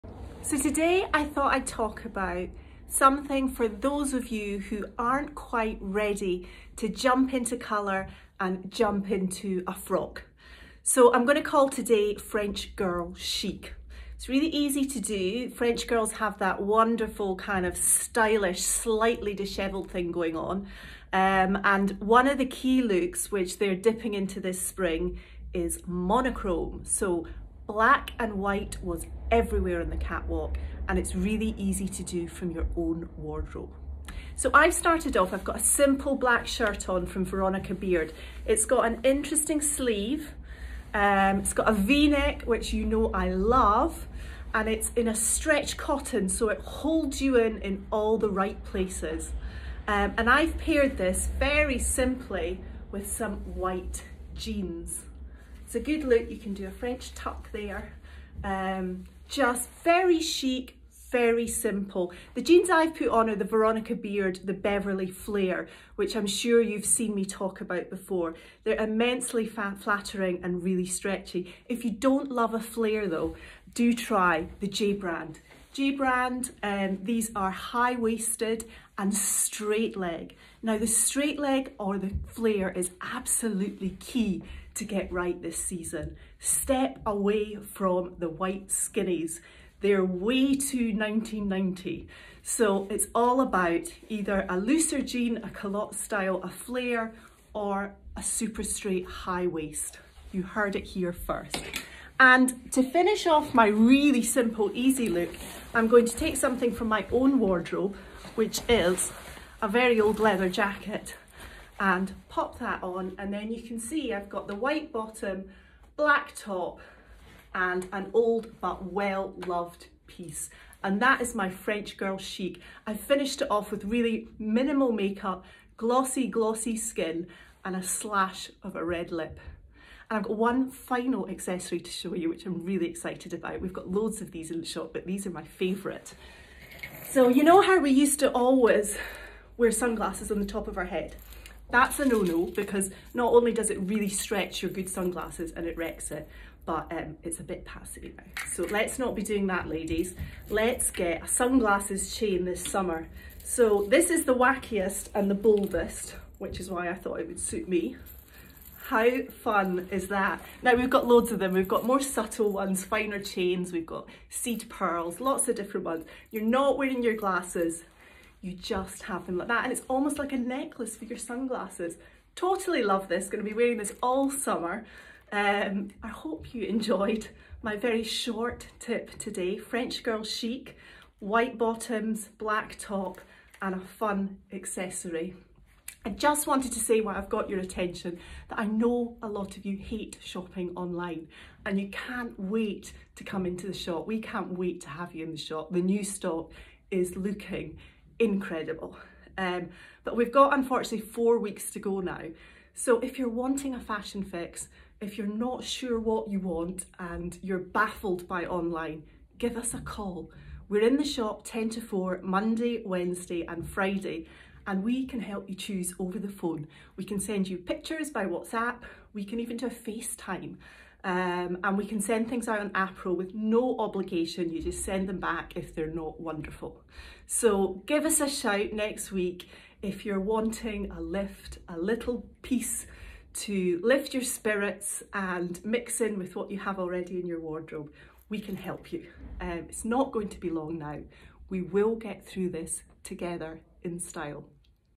French Girl Chic: Monochrome
This week I wanted to look into a key trend I seen on almost every runway for this season, monochrome! This black and white trend is for the people who aren't ready for colour yet. Something easy, casual and exceptionally chic.

Veronica Beard Black Bab Shirt £245
Veronica Beard Beverley, White £295
J Brand Ruby High Rise Cigarette, Blanc £230
Talis Chians Pin Necklace £55
Talis Chains Pearly Deluxe Sunglasses Chain £125
No.21 Black Aviator Sunglasses £205